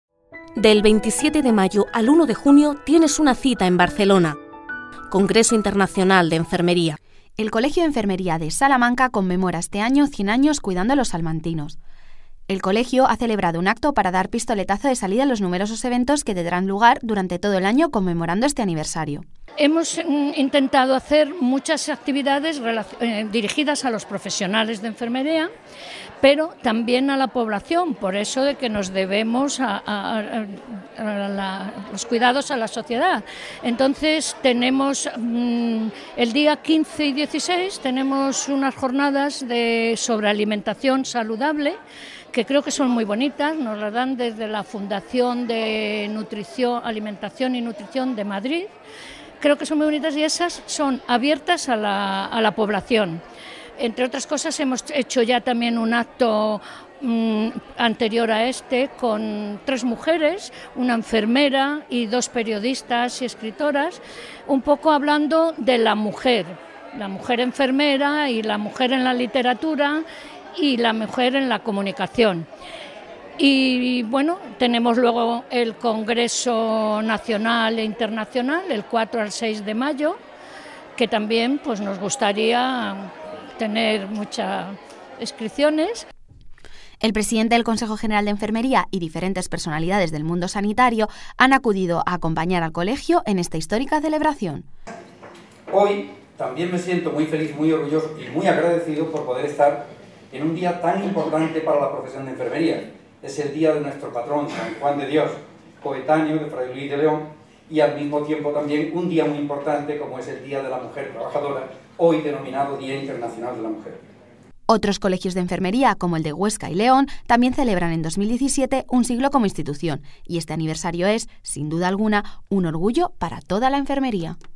Cien años cuidando a los salmantinos
Con ocasión del día de San Juan de Dios, el Colegio de Enfermería de Salamanca dio pistoletazo de salida a todos los eventos que tendrán lugar a lo largo de 2017 para conmemorar que la institución lleva 100 años a cargo de la salud de los ciudadanos de la provincia